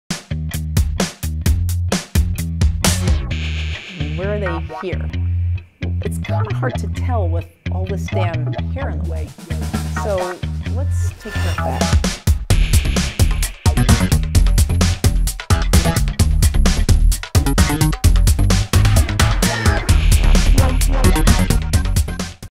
An MIT Professor Shaved Her Head For A Class
She wanted to show students where regions of the brain are.